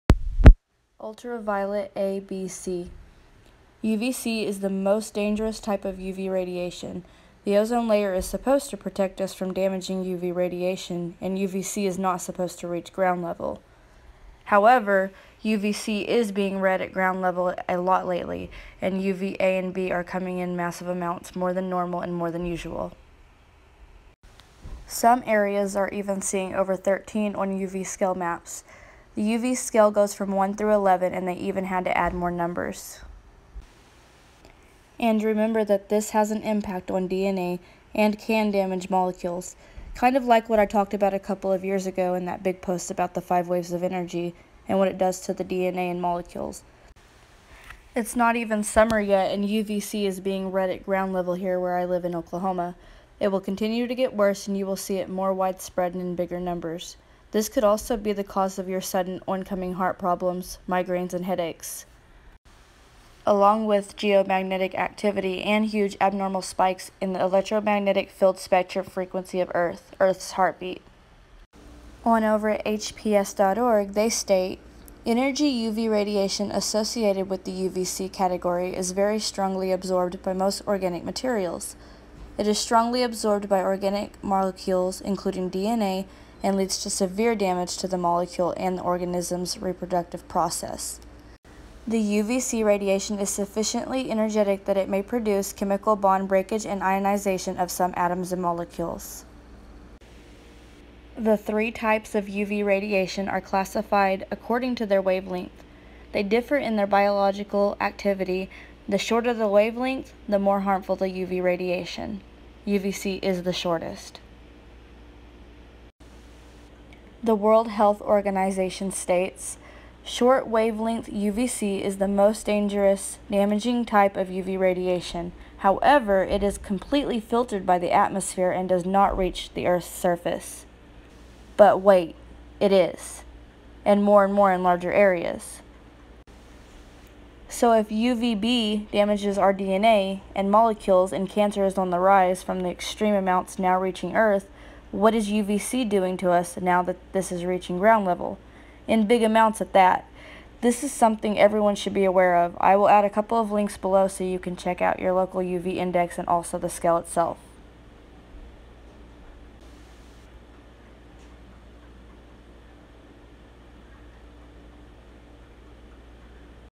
⚠️ UVC At Ground Level - UVC Explained 🚨
UV A/B/C - Ultraviolet A/B/C

UVC is the most dangerous type of UV Radiation. the ozone layer is SUPPOSED to protect us from damaging uv radiation
And UVC Is not supposed to reach ground level. However, UVC is being read at ground level a lot lately.  And UV A/B are coming in massive amounts more than normal and more than usual. Some areas were/is even seeing over 13 on UV maps!! The UV scale goes from 1 - 11 ..
They had to add more numbers .
Remember that this has an impact on DNA and can damage molecules, kind of like I talked about a couple years ago in that big post about “5 waves of energy” and what it does to the DNA and Molecules ..
it’s not even summer yet and UVC is being read at ground level here where I live in Oklahoma.
It will continue to get worse and you will see it more wide spread and in bigger numbers.
This could also be the cause of your sudden oncoming heart problems and migraines/Headaches along with Geomagnetic activity  and huge abnormal spikes in the Schumann resonance (Earths heartbeat-electromagnetic field spectrum frequency of planet earth)

“energy UV radiation associated with the UVC category is very strongly absorbed by most organic materials. The strong absorption by organic molecules, including DNA, leads to severe damage to the molecule and to the organism's reproductive processes, leading to the death of the microbes.
The UVC radiation is sufficiently energetic that individual photons may produce chemical bond breakage and ionization of some atoms and molecules”

UV info
The three types of UV radiation are classified according to their wavelength.
They differ in their biological activity.
 The shorter the wavelength, the more harmful the UV radiation. (UVC is shortest)

World Health Org - “Short-wavelength UVC is the most damaging type of UV radiation. However, it is completely filtered by the atmosphere and does not reach the earth's surface.” 🤔 but wait it is .. and more and more in larger areas 🤔

Here is a link (1.) to check local numbers for your area, and here is a link (2.) On what the scale and numbers mean
Remember to keep none toxic sun screen on hand

UV Index scale

A UV Index reading of 0 to 2 means low danger from the sun's UV rays for the average person.

A UV Index reading of 3 to 5 means moderate risk of harm from unprotected sun exposure.

A UV Index reading of 6 to 7 means high risk of harm from unprotected sun exposure. Protection against skin and eye damage is needed.

A UV Index reading of 8 to 10 means very high risk of harm from unprotected sun exposure. Take extra precautions because unprotected skin and eyes will be damaged and can burn quickly.

A UV Index reading of 11 or more means extreme risk of harm from unprotected sun exposure. Take all precautions because unprotected skin and eyes can burn in minutes.

Remember it is now reaching over 13 in some areas

- Laura Wells